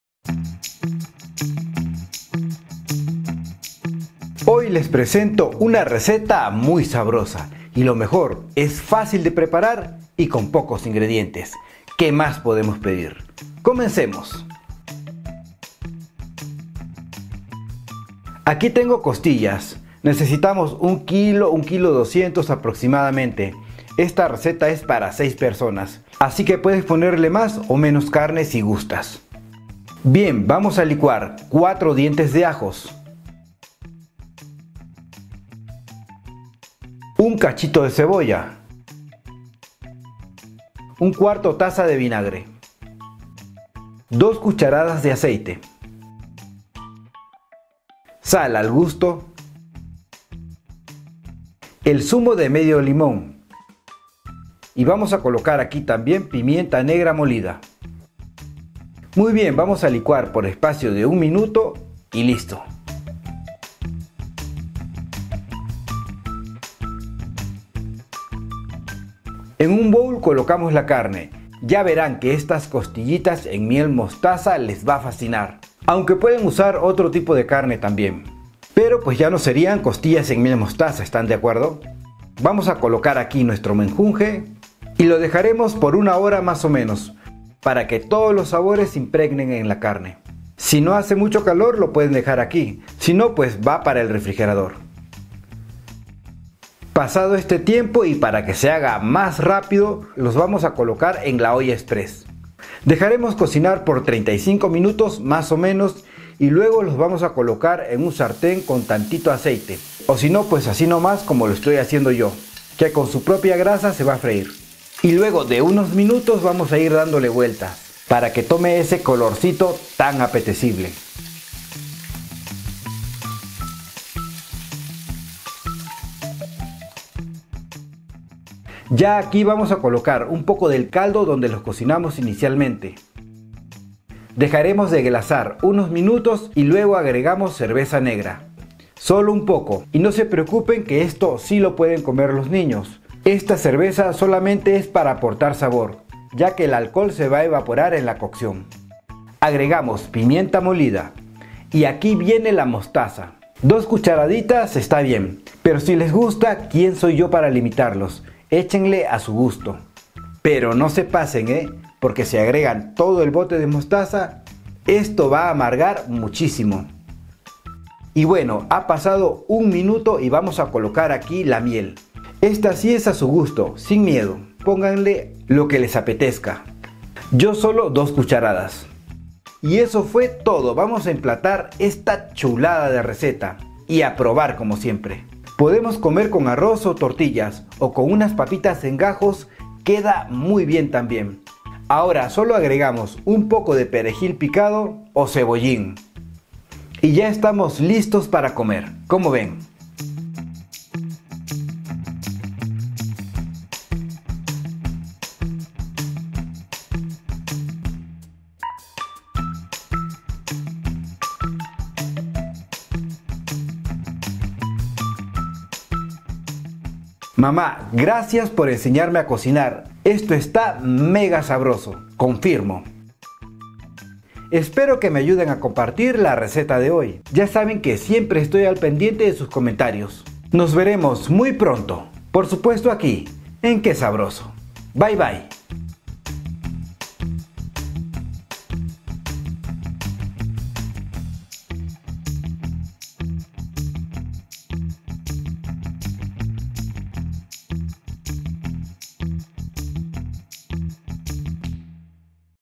Sabrosas Costillitas a la Miel Mostaza | Receta SIN HORNO
Ingredientes:
1 kilo de costillas de cerdo
4 dientes de ajos
1 trozo de cebolla
1/4 taza de vinagre
2 cucharadas de aceite
Zumo de medio limón
Sal y pimienta al gusto
2 cucharadas de mostaza
1 taza de cerveza negra
3 cucharadas de miel.

¡Buen provecho!

¿Quieres invitarme un licuado de frutas?

Hey ¿Qué haces hasta acá abajo? los likes están arriba ↑↑↑